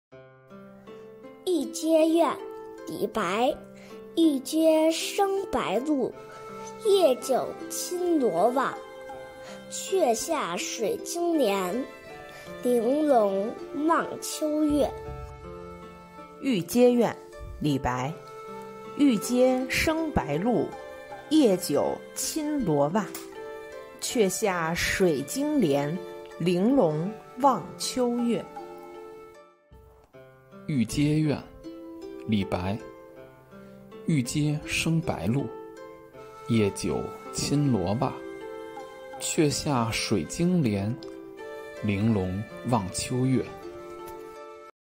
和萌娃每天一首诗-玉阶怨
和萌娃每天一首诗
第201首诗：玉阶怨-李白
玉阶生白露，夜久侵罗袜。
却下水晶帘，玲珑望秋月。